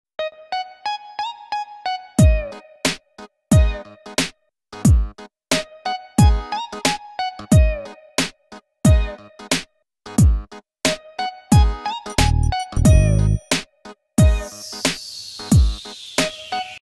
Restock: You know you love something when you have a whole wine fridge for it. 😅
You know you love Powerade when you have a whole wine fridge for it. 😅 💙💜❤️

Now before all of the comments come rolling in… this was a had me down, so why not use it for something we love and actually drink?! 🤷🏼‍♀️😂

Check out my LTK page for even more of my favorite projects from even MORE brands